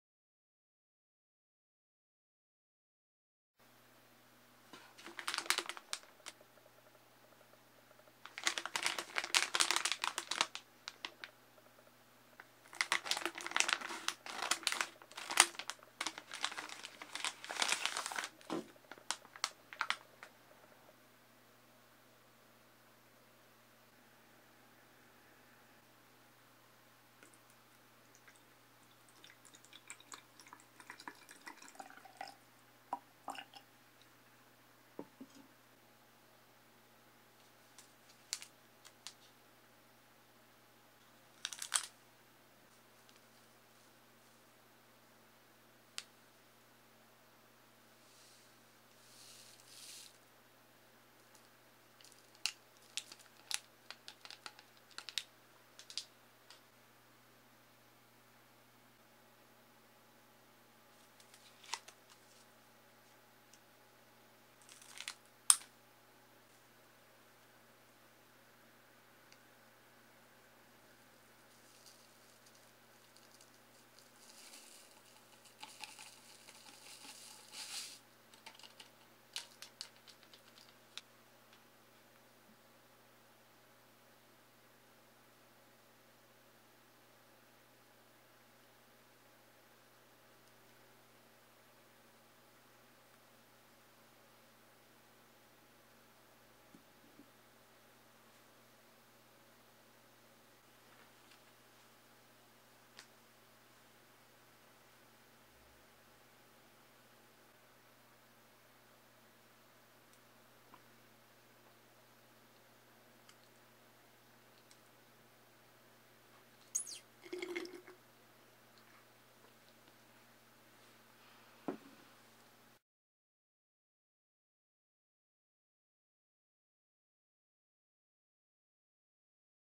포핀쿠킨 - 짱구 실험실 드링크 / popin cookin - Crayon Shin Chan Experimental Drink
안녕하세요 미고입니다~^▽^)/♪
간편하고 맛도 좋은(?) 실험실 드링크에요
거품이 촤~하 하고 올라올줄 알았는데... 조금 올라오다 끝나네여 ㅠ

맛은 콜라맛이라고 써있구요 포쿠 햄버거에 들어있는 콜라랑 똑같은 맛인거 같았어요
제입엔 콜라맛보단 포도맛?? 비슷하게 느껴졌다눈..
탄산이 많이 느껴져서 나름 맛있게 먹었네요

물은 100cc넣으시면 됩니다
그럼 오늘도 즐감하세요♡